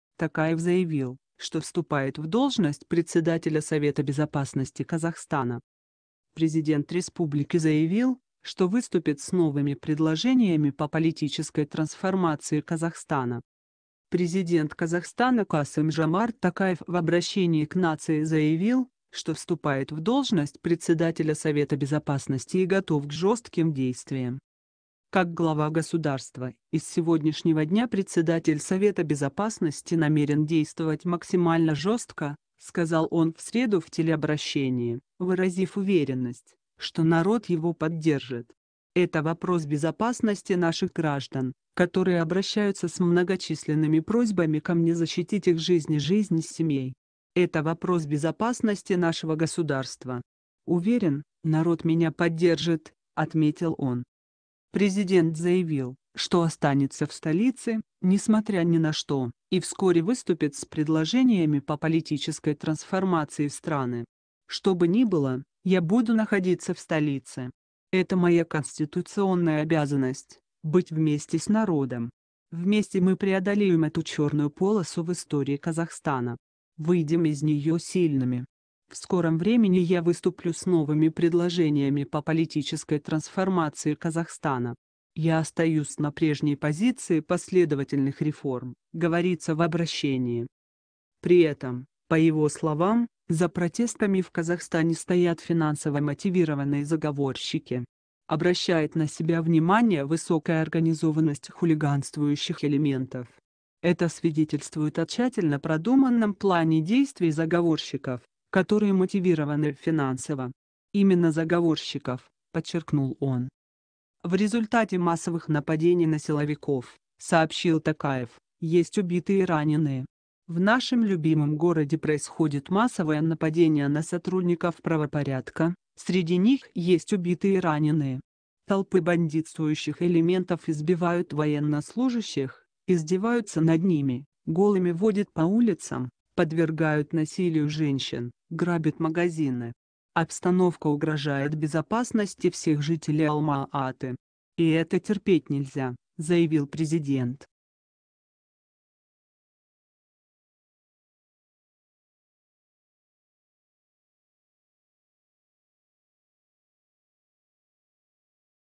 Токаев заявил, что вступает в должность председателя Совета безопасности Казахстана
Президент республики заявил, что выступит с новыми предложениями по политической трансформации Казахстана.
Президент Казахстана Касым-Жомарт Токаев в обращении к нации заявил, что вступает в должность председателя Совета безопасности и готов к жестким действиям.

"Как глава государства, и с сегодняшнего дня председатель Совета безопасности намерен действовать максимально жестко", - сказал он в среду в телеобращении, выразив уверенность, что народ его поддержит. "Это вопрос безопасности наших граждан, которые обращаются с многочисленными просьбами ко мне защитить их жизнь и жизнь семей. Это вопрос безопасности нашего государства. Уверен, народ меня поддержит", - отметил он.

Президент заявил, что останется в столице, несмотря ни на что, и вскоре выступит с предложениями по политической трансформации страны. "Что бы ни было, я буду находиться в столице. Это моя конституционная обязанность - быть вместе с народом. Вместе мы преодолеем эту черную полосу в истории Казахстана. Выйдем из нее сильными. В скором времени я выступлю с новыми предложениями по политической трансформации Казахстана. Я остаюсь на прежней позиции последовательных реформ", - говорится в обращении.

При этом, по его словам, за протестами в Казахстане стоят "финансово мотивированные заговорщики". "Обращает на себя внимание высокая организованность хулиганствующих элементов. Это свидетельствует о тщательно продуманном плане действий заговорщиков, которые мотивированы финансово. Именно заговорщиков", - подчеркнул он.

В результате массовых нападений на силовиков, сообщил Токаев, есть убитые и раненые. "В нашем любимом городе происходят массовые нападения на сотрудников правопорядка, среди них есть убитые и раненые. Толпы бандитствующих элементов избивают военнослужащих, издеваются над ними, голыми водят по улицам, подвергают насилию женщин, грабят магазины. Обстановка угрожает безопасности всех жителей Алма-Аты. И это терпеть нельзя", - заявил президент.